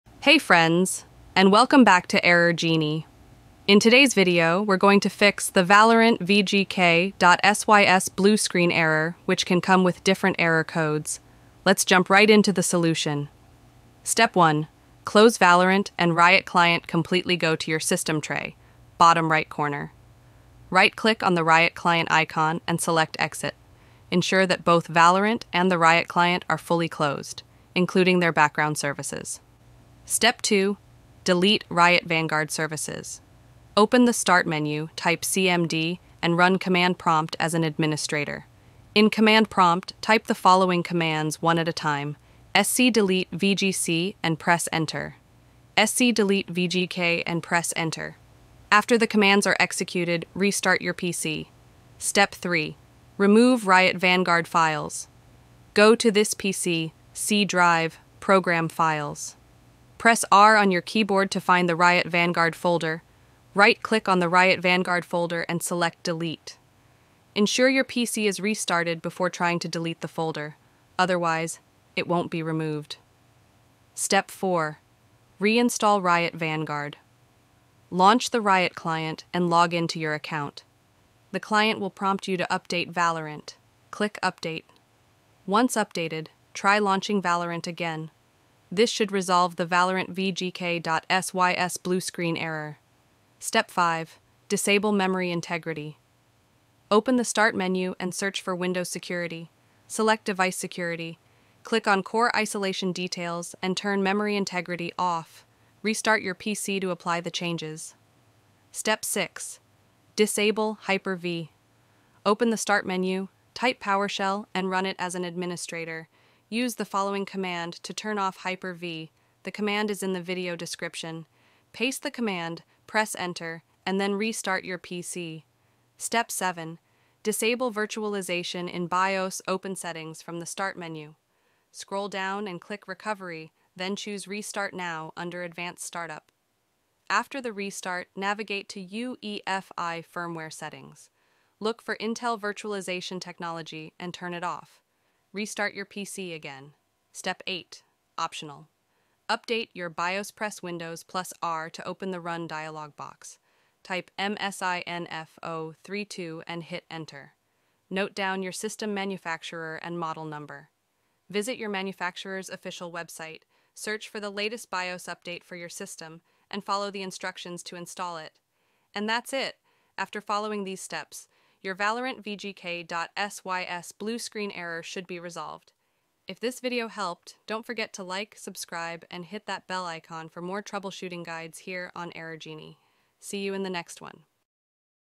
Fix Valorant VGK.SYS Blue Screen Error Windows 10 11 in 8 Easy Steps
Fix Valorant VGK.SYS Blue Screen Error Windows 10 11 in 8 Easy Steps. Hey friends, and welcome back to Error Genie! In today’s video, we’re going to fix the Valorant VGK.SYS Blue screen error, which can come with different error codes. Let’s jump right into the solution!

Step 1: Close Valorant and Riot Client Completely
Go to your System Tray (bottom right corner).
Right-click on the Riot Client icon and select Exit.
Ensure that both Valorant and the Riot Client are fully closed, including their background services.

Step 2: Delete Riot Vanguard Services
Open the Start menu, type CMD, and run Command Prompt as an Administrator.
In Command Prompt, type the following commands one at a time:
sc delete vgc and press Enter.
sc delete vgk and press Enter.
After the commands are executed, restart your PC.

Step 3: Remove Riot Vanguard Files
Go to This PC - C: Drive - Program Files.
Press R on your keyboard to find the Riot Vanguard folder.
Right-click on the Riot Vanguard folder and select Delete.
(Ensure your PC is restarted before trying to delete the folder; otherwise, it won’t be removed).

Step 4: Reinstall Riot Vanguard
Launch the Riot Client and log in to your account.
The client will prompt you to update Valorant. Click Update.
Once updated, try launching Valorant again. This should resolve the Valorant VGK.SYS Blue screen error.

Step 5: Disable Memory Integrity
Open the Start menu and search for Windows Security.
Select Device Security.
Click on Core Isolation Details and turn Memory Integrity Off.
Restart your PC to apply the changes.

Step 6: Disable Hyper-V
Open the Start menu, type PowerShell, and run it as an Administrator.
Use the following command to turn off Hyper-V (the command is in the video description):
Paste the command, press Enter, and then restart your PC.

command to paste in powershell:
bcdedit /set hypervisorlaunchtype off

Step 7: Disable Virtualization in BIOS
Open Settings from the Start menu.
Scroll down and click Recovery, then choose Restart Now under Advanced Startup.
After the restart, navigate to UEFI Firmware Settings.
Look for Intel Virtualization Technology and turn it Off.
Restart your PC again.

Step 8: (Optional) Update Your BIOS
Press Windows + R to open the Run dialog box.
Type msinfo32 and hit Enter.
Note down your System Manufacturer and Model Number.
Visit your manufacturer’s official website, search for the latest BIOS update for your system, and follow the instructions to install it.

And that’s it! After following these steps, your Valorant VGK.SYS Blue screen error should be resolved.